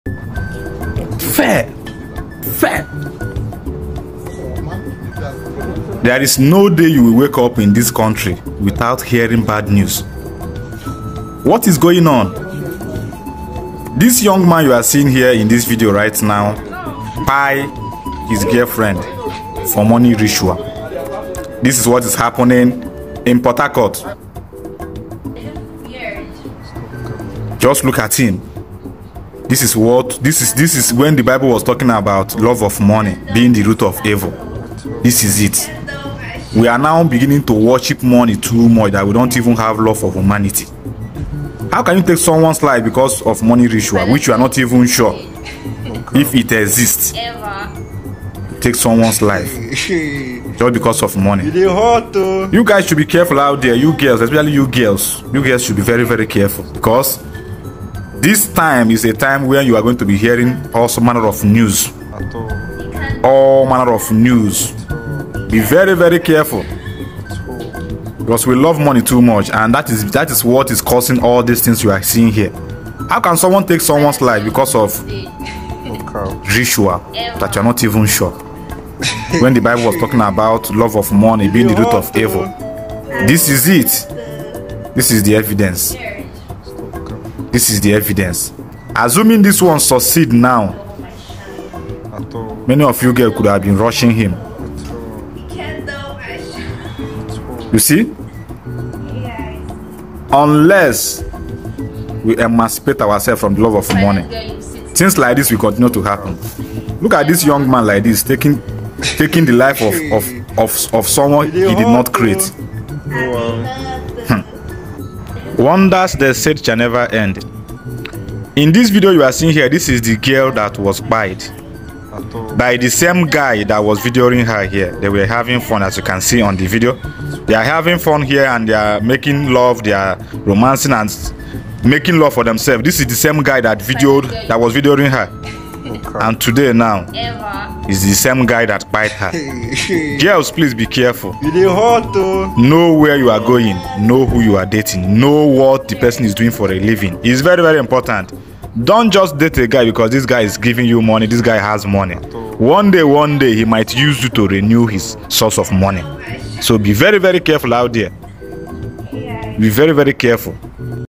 Justina who was k!lled by her r!tualist boyfriend in uniport
Justina who was k!lled by her ritualist boyfriend in uniport